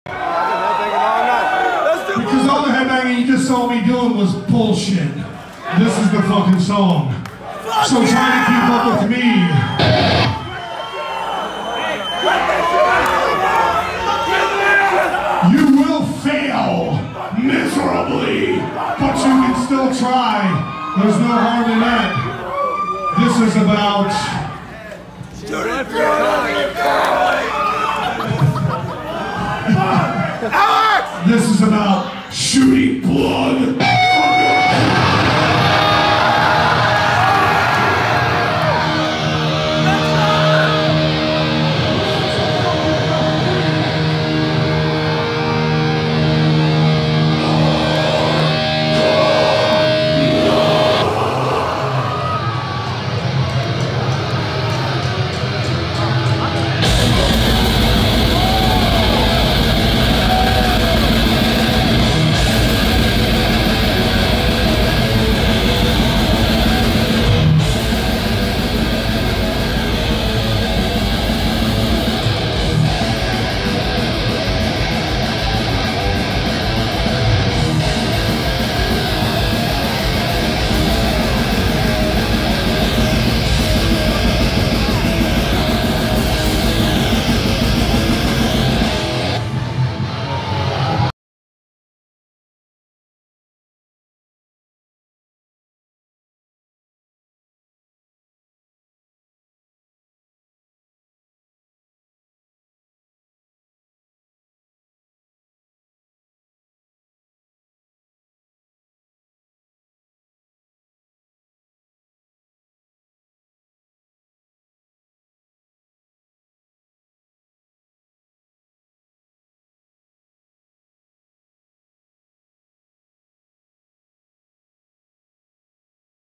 The Ultimate Corpsegrinder Windmill Headbang
0:55 Cannibal Corpse' first show of the 2019 North American Tour with Thy Art Is Murder, and Perdition Temple 10/20/19